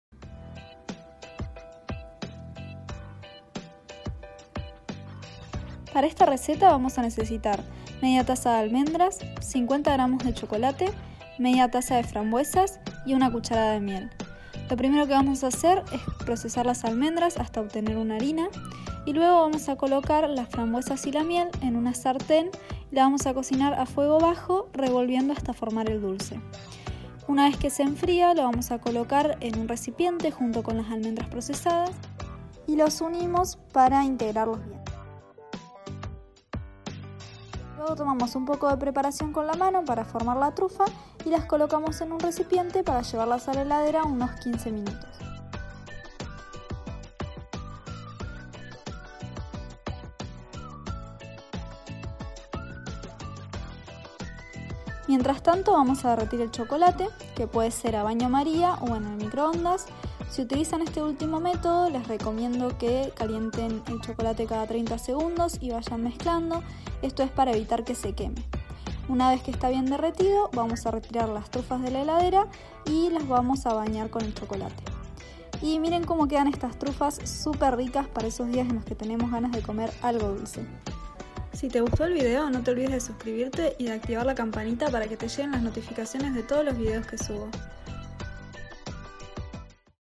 TRUFAS SALUDABLES
Hoy les quiero compartir esta receta de trufas, una opción distinta para esos días que queremos comer algo dulce. Riquísimas y fáciles de hacer.